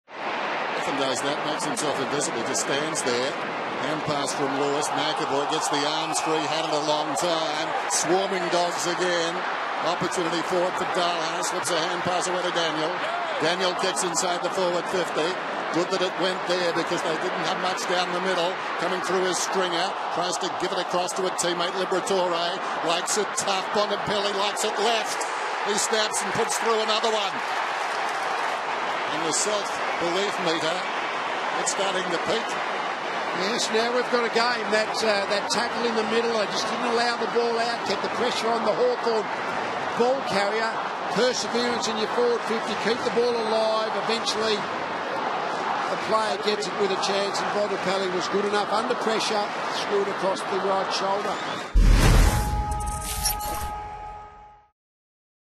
Bontempelli finishes sweetly - AFL
Marcus Bontempelli kicks a lovely goal against the Hawks.